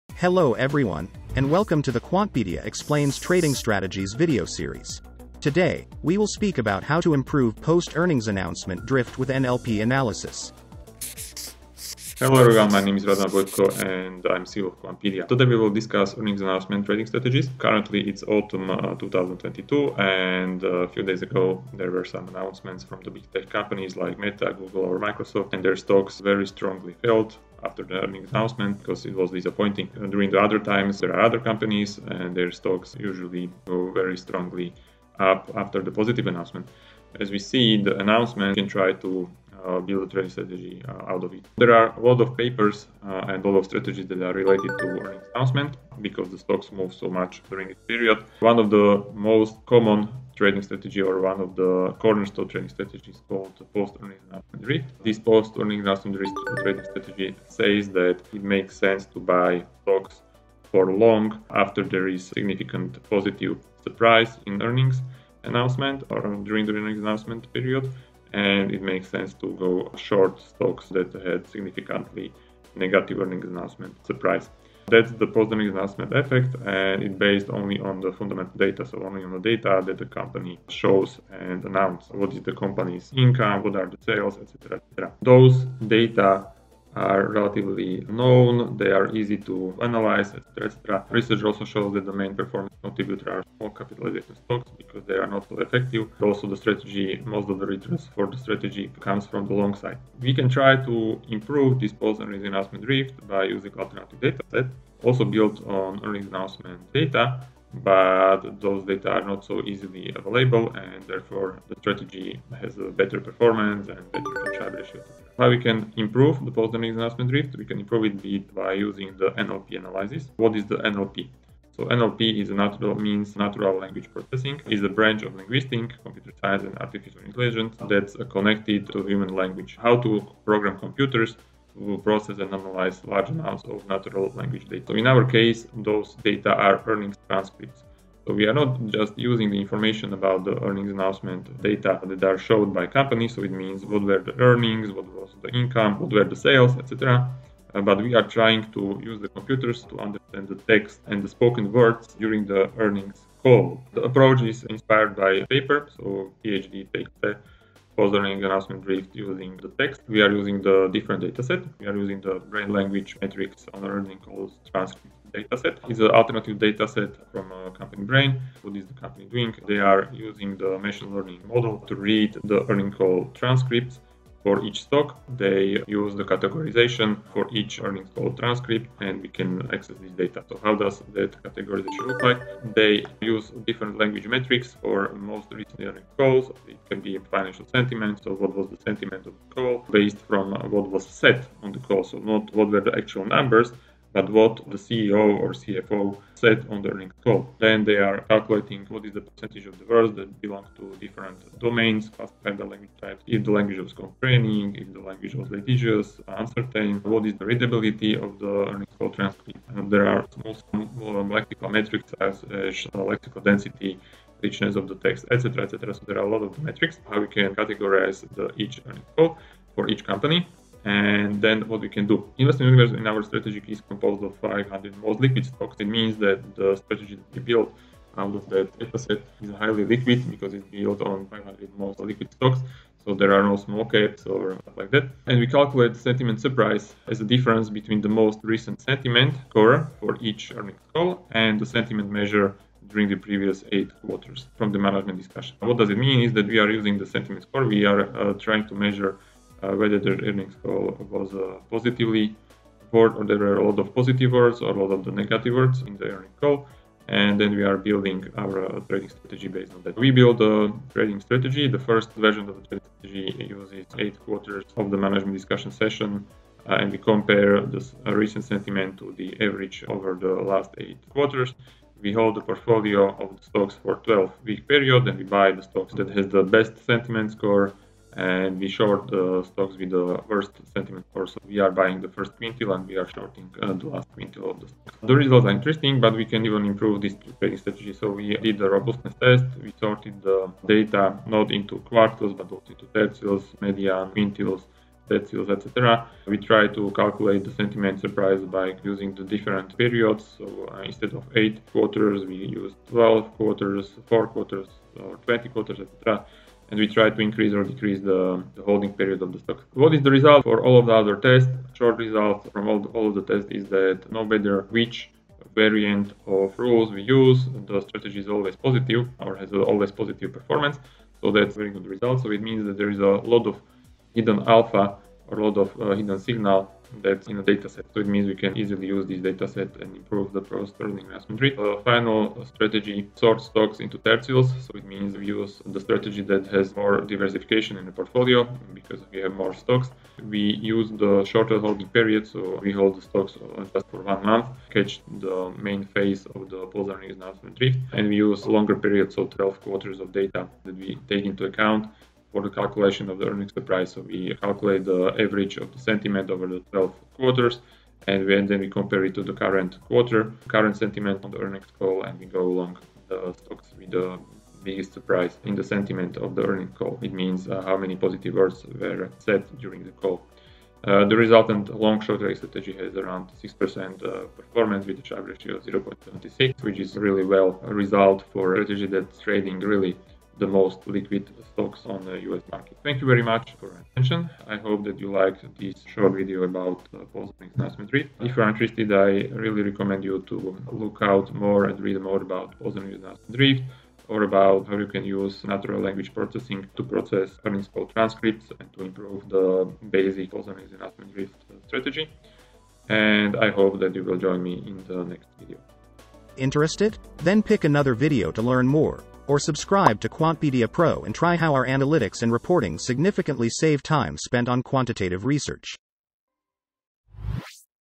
Post Earnings Announcement Drift with NLP Analysis - Quantpedia Explains (Trading Strategies)
This video will speak about the Cross Sectional Mean Reversion Trading Strategies, introduced by the CEO of Quantpedia.com, Radovan Vojtko.